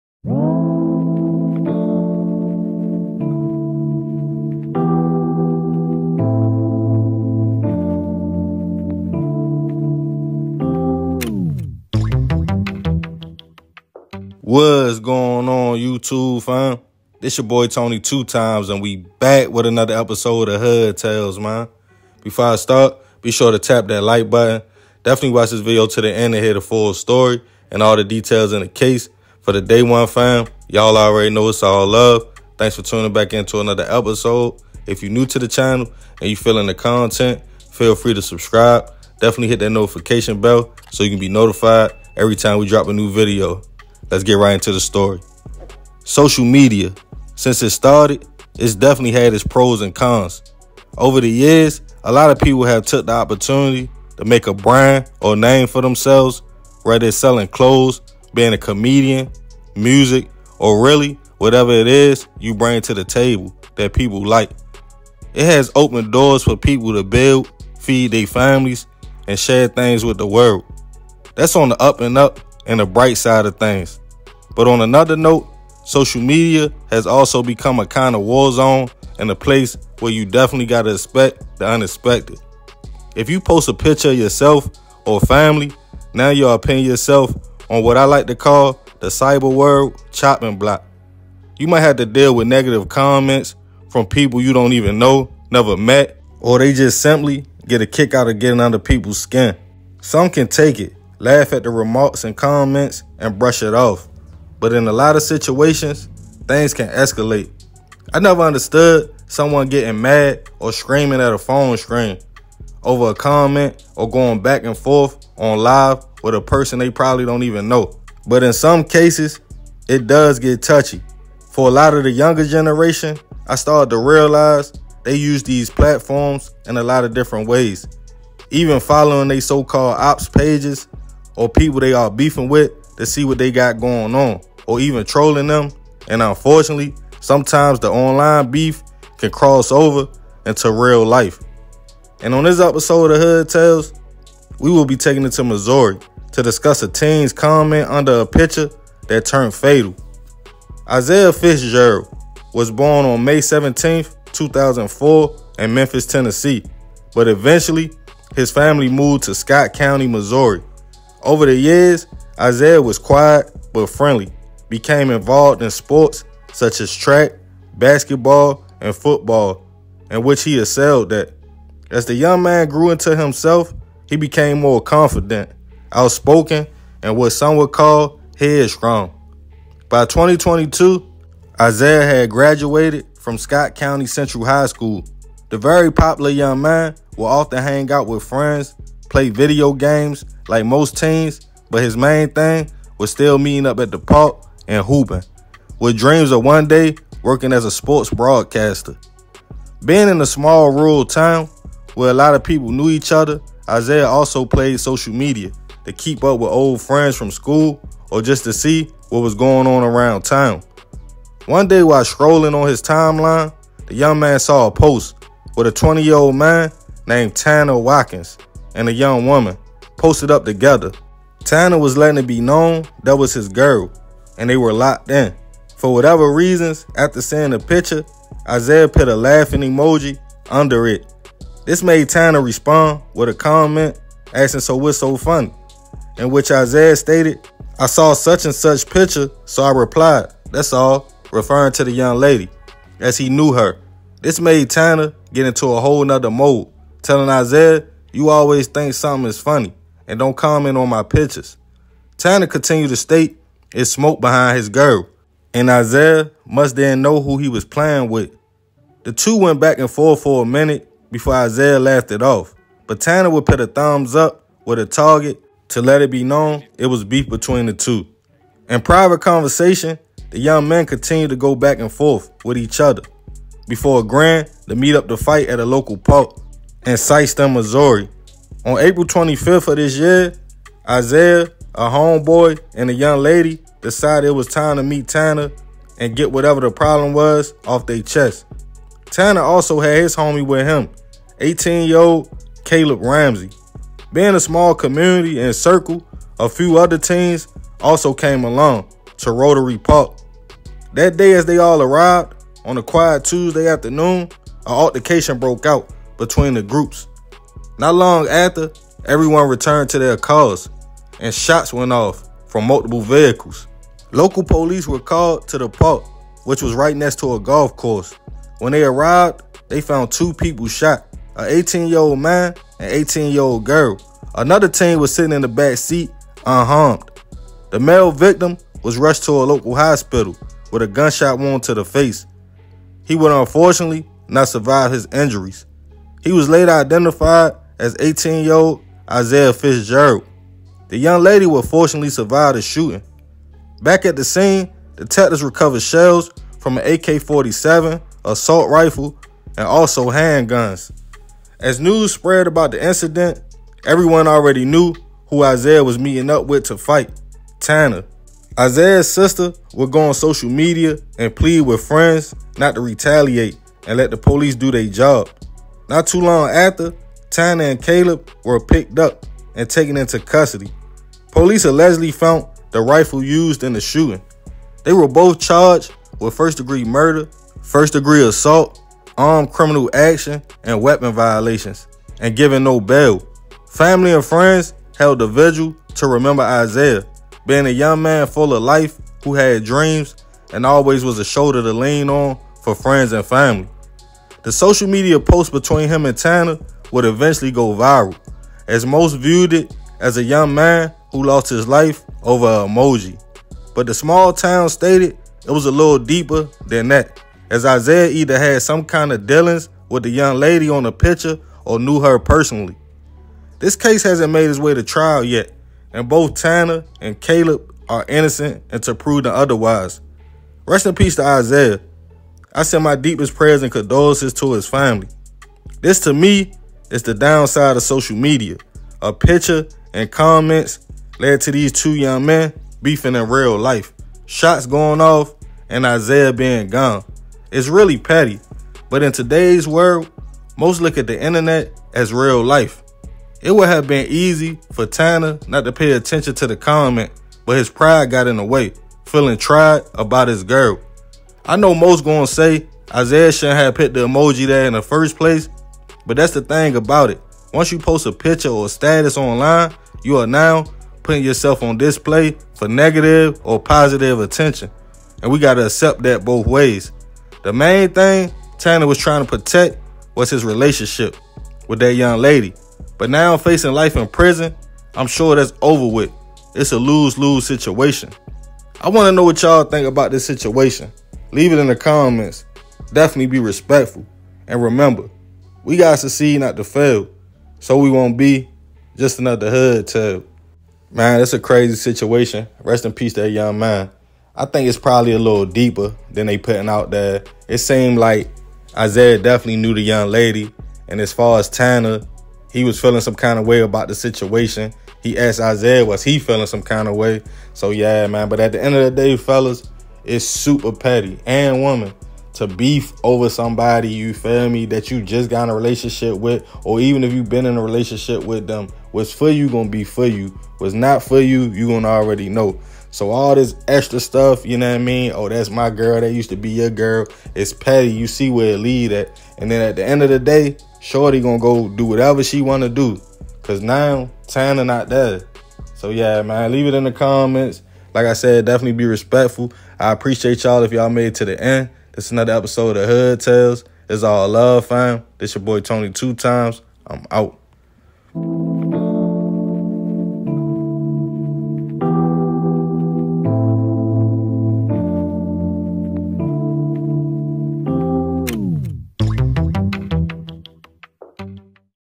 Teen Loses Life Over Comment Under Post With Man & His Girlfriend
On this episode of Hood Tales we will be taking it to Sikeston Missouri to discuss the case of Isiah Fitzgerald a teen who lost his life over a comment under a post with a man and his girlfriend.